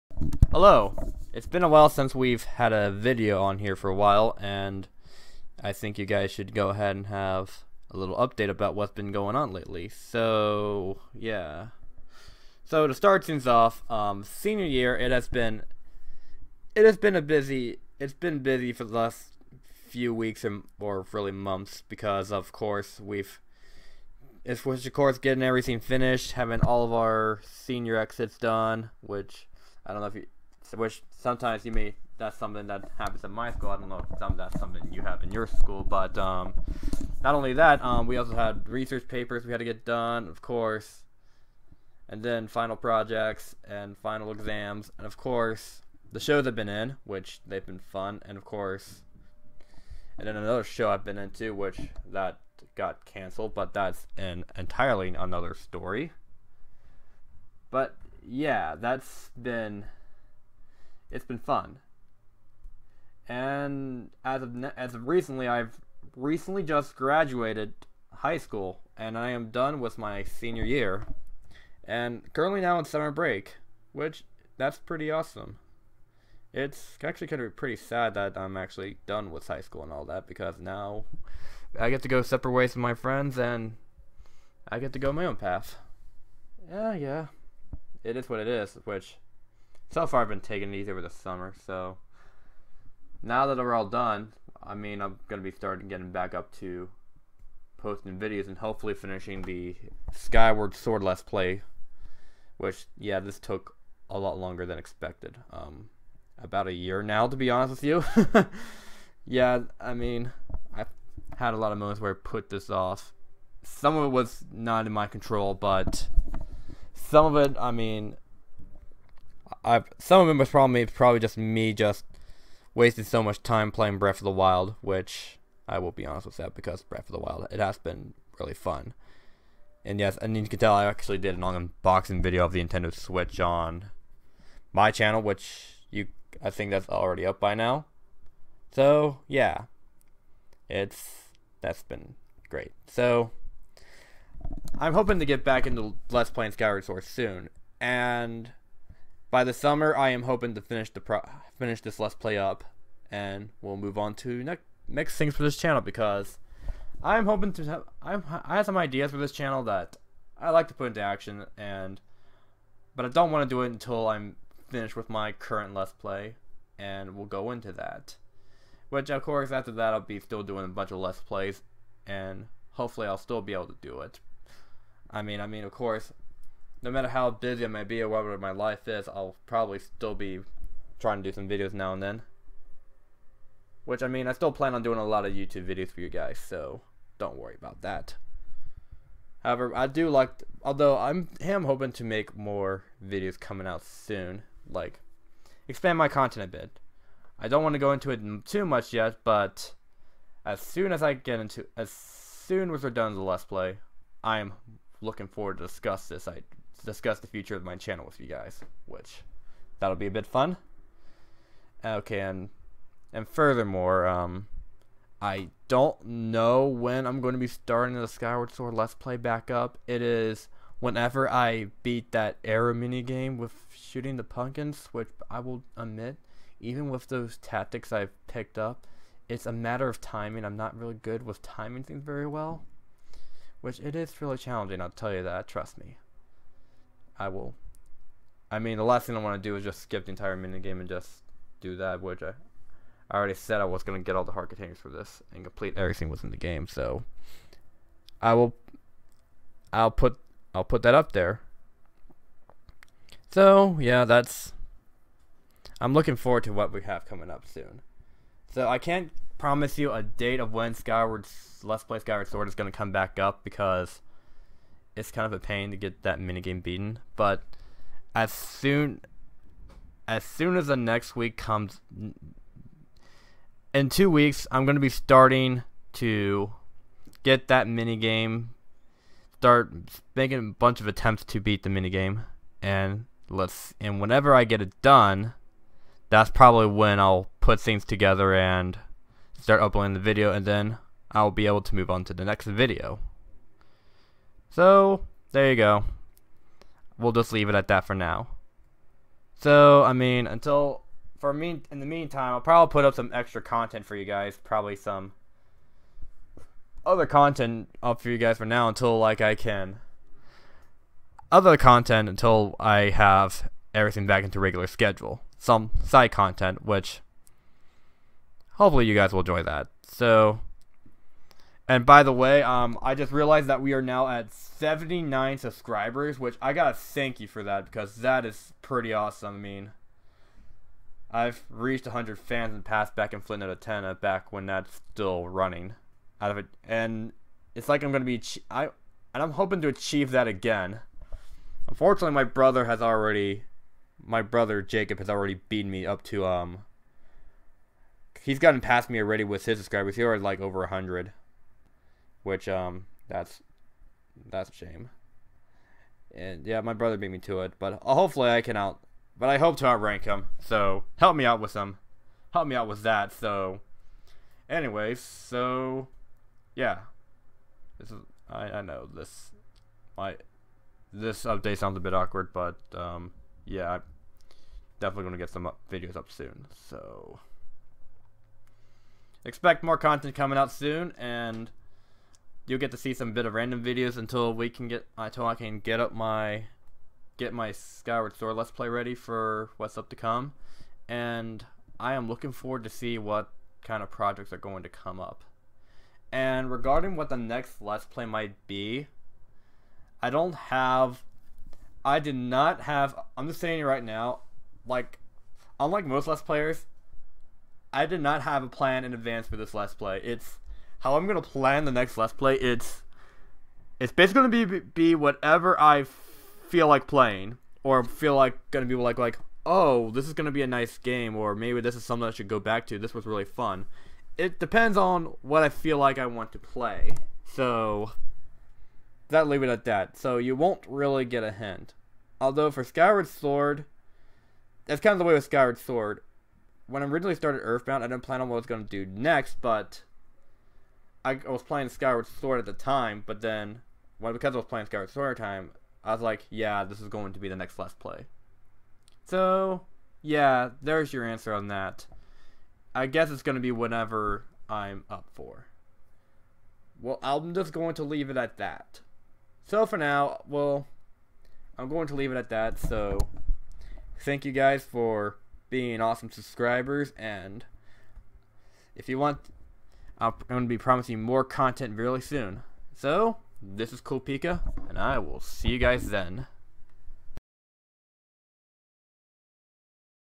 Update 6 9 2017
I think it's about time I had an update video. I got the whole summer to get videos out to you  I'm hoping to finish the lets play somewhere this summer hopefully.